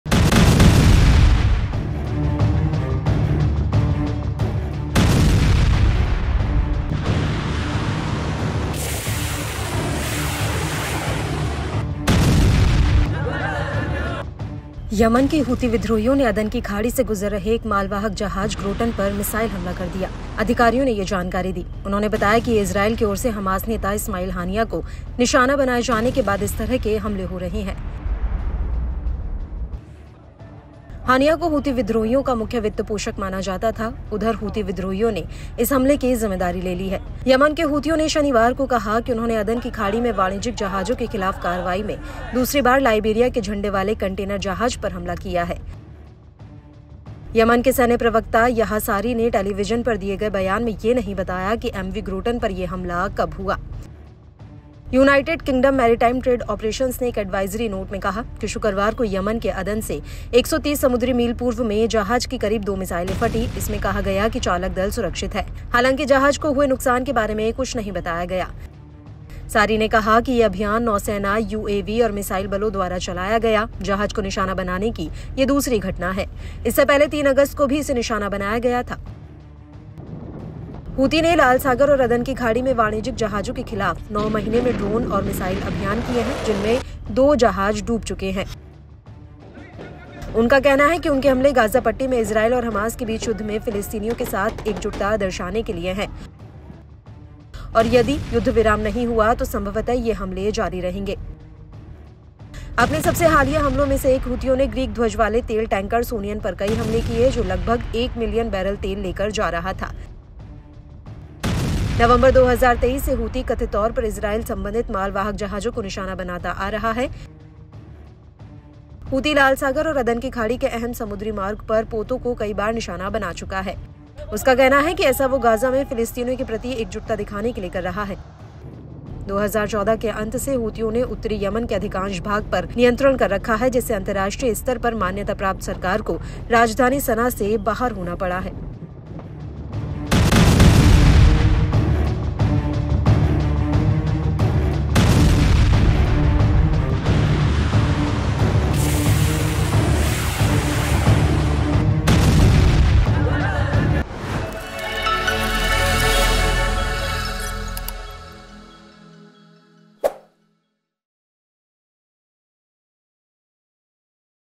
Yemeni Armed Forces Attack: हूतियों ने दूसरी बार कंटेनर पोत पर किया हमला | Israel | Hamas
यमन के हूती विद्रोहियों ने अदन की खाड़ी से गुजर रहे एक मालवाहक जहाज पर शनिवार देर रात मिसाइल हमला कर दिया। अधिकारियों ने रविवार को यह जानकारी दी।
Yemen's Houthi rebels launched a missile attack on a cargo ship passing through the Gulf of Aden late Saturday night. Officials gave this information on Sunday.